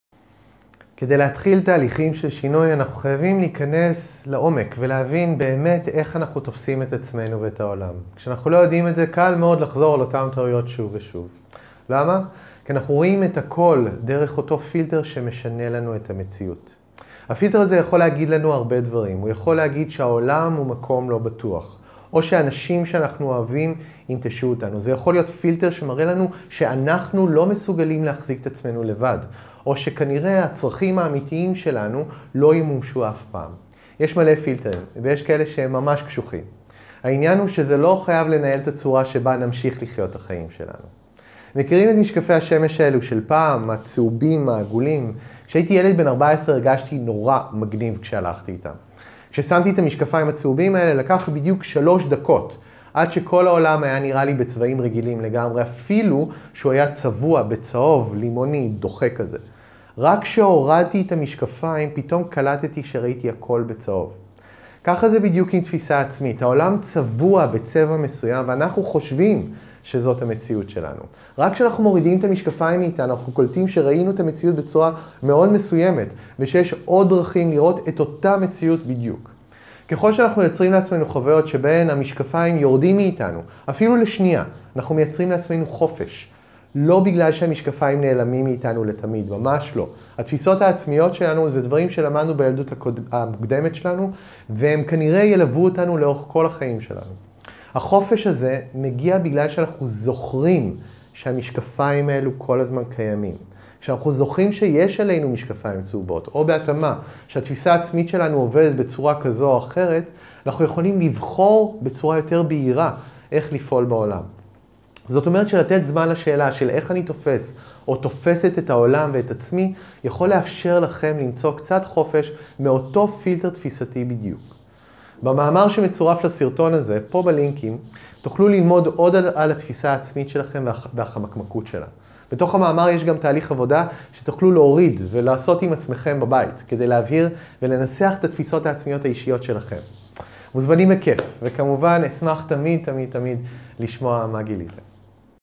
פילטרים של מציאות: תפיסה עצמית
כדי להתחיל תהליכים של שינוי, אנחנו חייבים להכנס לעומק ולהבין באמת איך אנחנו תופסים את עצמנו ואת העולם. כשאנחנו לא יודעים מה התפיסות שלנו לגבי עצמנו ולגבי העולם, קל מאוד לחזור על אותן טעויות שוב ושוב.